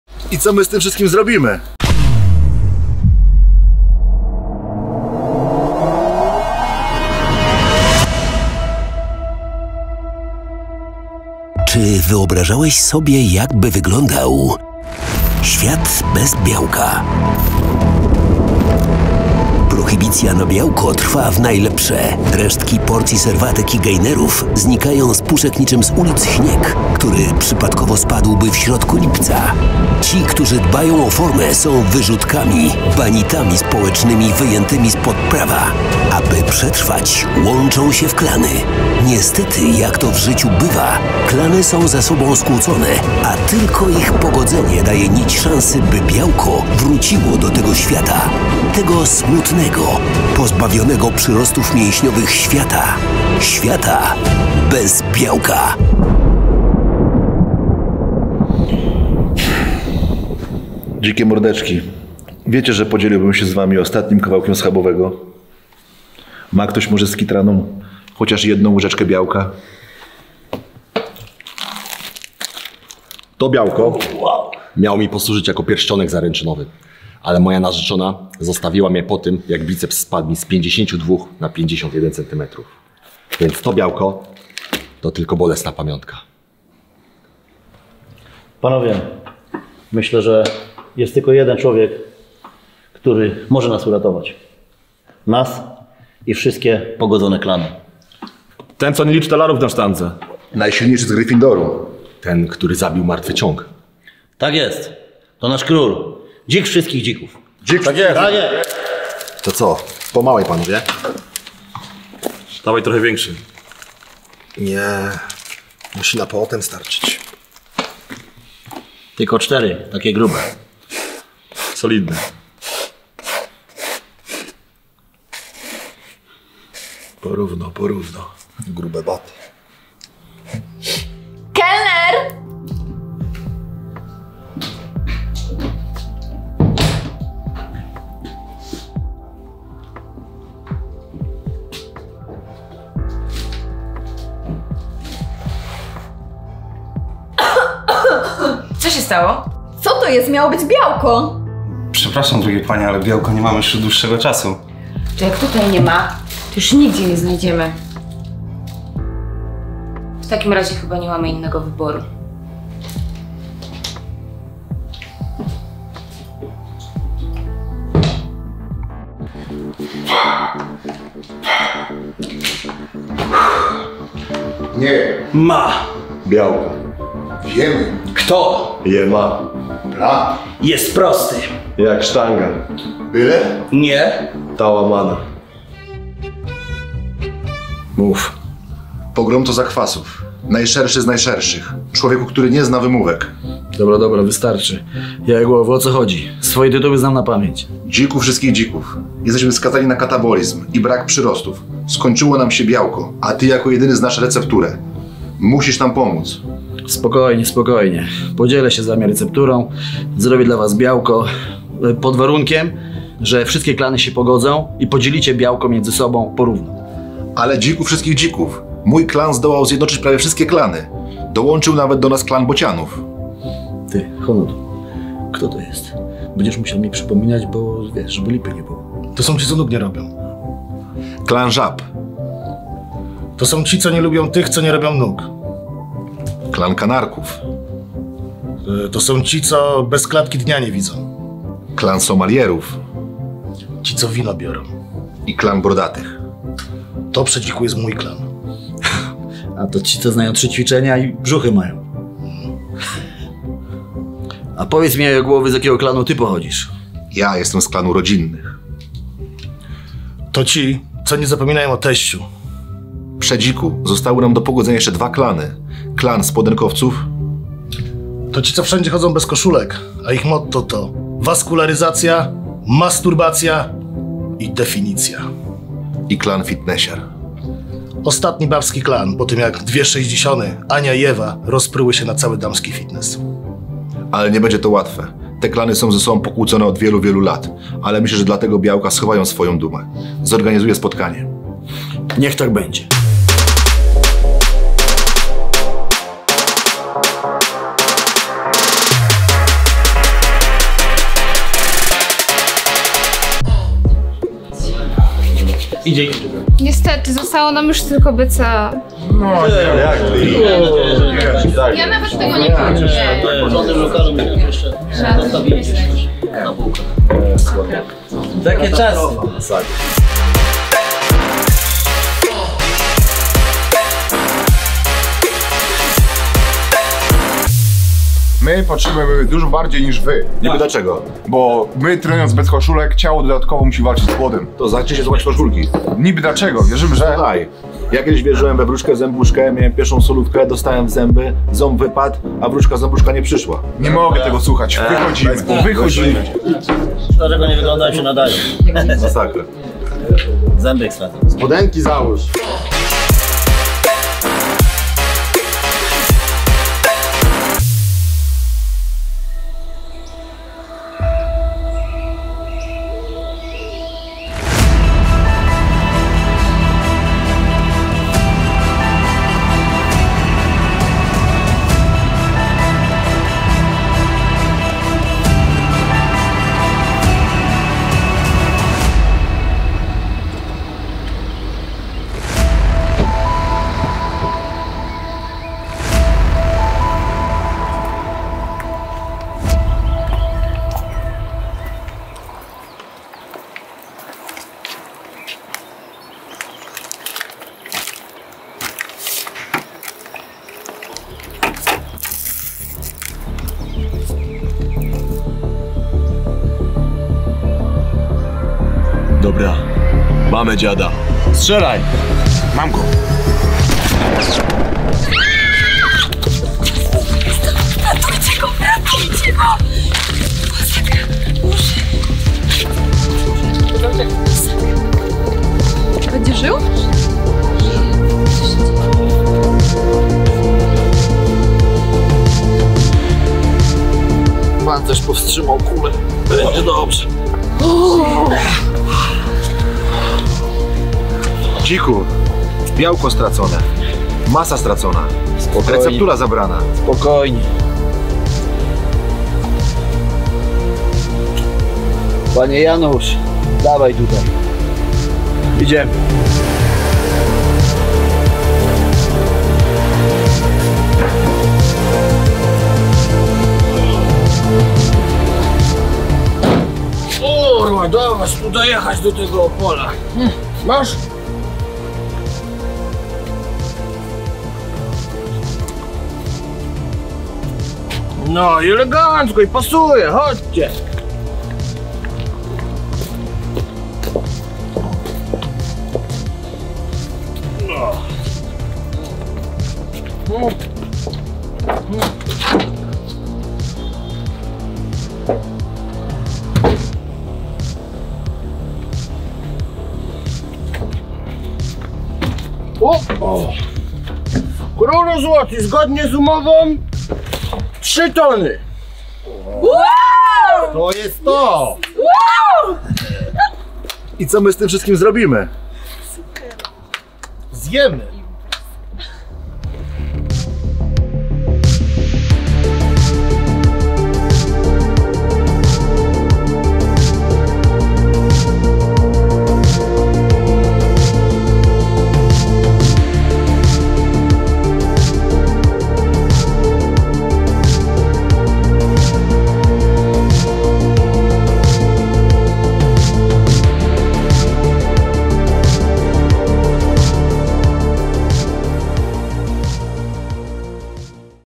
ŚWIAT BEZ BIAŁKA - SFD Comedy
Czy wyobrażałeś sobie kiedyś jakby wyglądał świat...

Prohibicja na białko trwa w najlepsze. Resztki porcji serwatek i gainerów  znikają z puszek niczym z ulic śnieg, który przypadkowo spadłby w środku lipca. Ci, którzy dbają o formę są wrzutkami, banitami społecznymi wyjętymi spod prawa. Aby więc przetrwać łączą się w klany.

Niestety jak to w życiu bywa klany są ze sobą skłócone a tylko ich pogodzenie daje nić szansy by białko wróciło do tego świata...
Tego smutnego, pozbawionego przyrostów mięśniowych świata...

...ŚWIATA BEZ BIAŁKA.

Jeśli nie wyobrażasz sobie jakby wyglądał świat bez białka to wbijaj na Sklep SFD i zrób zakupy już dzisiaj!
W filmie wystąpili:

Radosław Słodkiewicz
Krzysztof Piekarz
Maciej Żochowski
Agnieszka Cichosz
Szymon Madej
Żaklina Stańczyk
Dominika Bilska
Adam Piwko
Bartek Kubiak
Adrian Marcheczyński
Michał Tybora
Patryk Jepiszko
Rafał Tasiemski